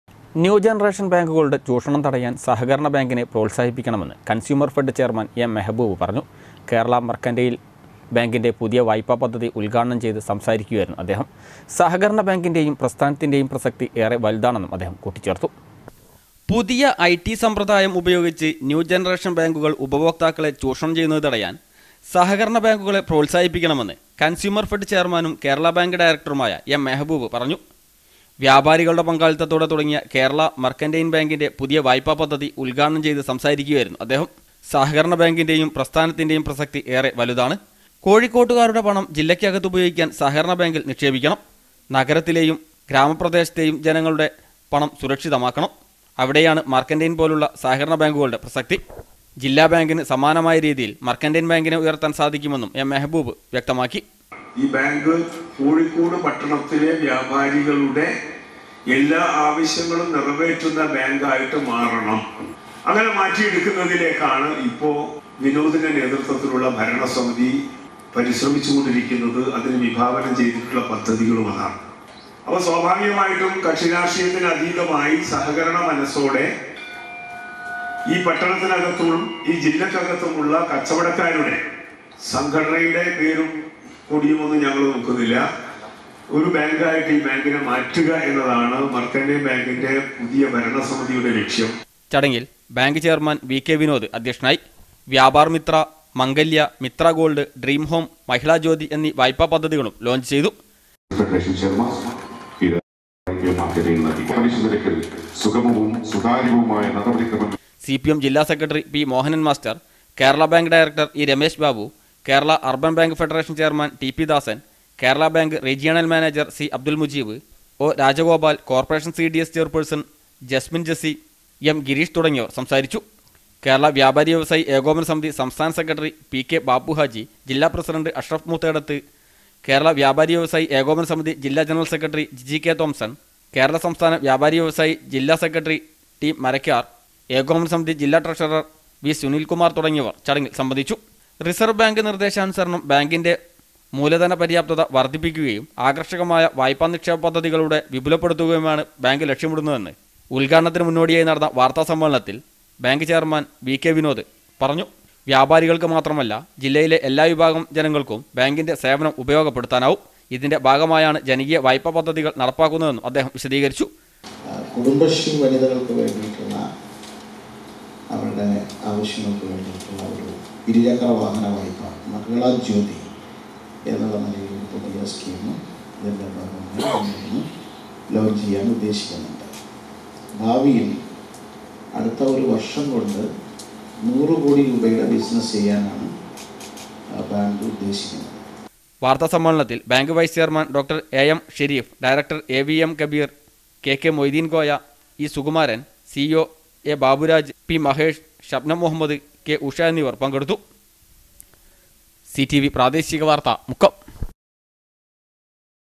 ന്യൂ ജനറേഷൻ ബാങ്കുകളുടെ ചൂഷണം തടയാൻ സഹകരണ ബാങ്കിനെ പ്രോത്സാഹിപ്പിക്കണമെന്ന് കൺസ്യൂർ ഫെഡ്
ന്യൂ ജനറേഷൻ ബാങ്കുകളുടെ ചൂഷണം തടയാൻ സഹകരണ ബാങ്കിനെ പ്രോത്സാഹിപ്പിക്കണമെന്ന് കൺസ്യൂർ ഫെഡ് ചെയർമാൻ എം മെഹബൂബ് പറഞ്ഞു.കേരള മെർക്കന്റയിൽ ബാങ്കിന്റെ പുതിയ വായ്പ പദ്ധതി ഉദ്ഘാടനം ചെയ്ത് സംസാരിക്കുകയായിരുന്നു അദ്ദേഹം.
സഹകരണ ബാങ്കിന്റെയും പ്രസ്ഥാനത്തിന്റെയും പ്രസക്തി ഏറെ വലുതാണന്നും അദ്ധേഹം കൂട്ടിച്ചേർത്തു